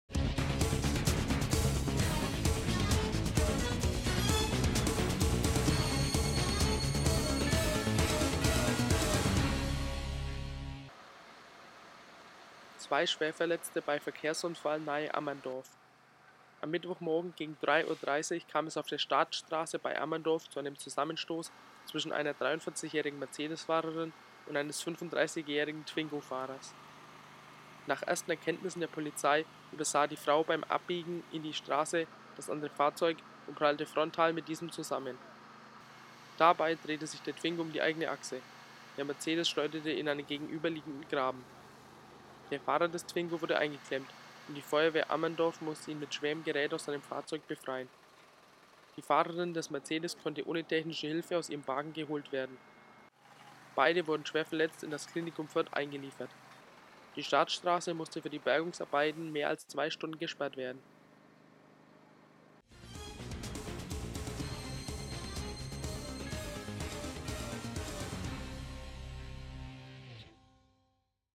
15.01.2014 (FÜ) Schwerer Verkehrsunfall nahe Ammerndorf
Gegen 3.30 Uhr fuhr die 43-jährige Mercedesfahrerin auf die Staatsstraße. Der 35-jährige Fahrer des Twingo konnte nicht mehr ausweichen und prallte frontal in den anderen Wagen. Dabei drehte sich der Twingo um die eigene Achse, der Mercedes schleuderte in einen gegenüberliegenden Graben.

Der Fahrer des Twingo wurde eingeklemmt und die Feuerwehr Ammerndorf musste ihn mit schwerem Gerät aus seinem Fahrzeug befreien. Die Fahrerin des Mercedes konnte ohne technische Hilfe aus ihrem Wagen geholt werden. Beide wurden schwer verletzt in das Klinikum Fürth eingeliefert. Die Staatsstraße musste für die Bergungsarbeiten mehr als zwei Stunden gesperrt werden. Die Feuerwehr leitete den Verkehr zeitweise durch Ammerndorf.